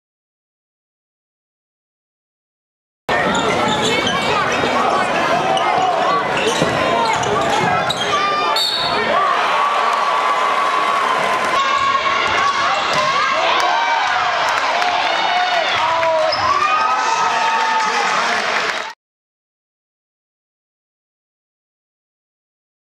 PrepBallersTV.com - James Harden Highlights
Highlight Video of James Harden, Class of 2007 - ASU. See the full video at PrepBallersTV.com.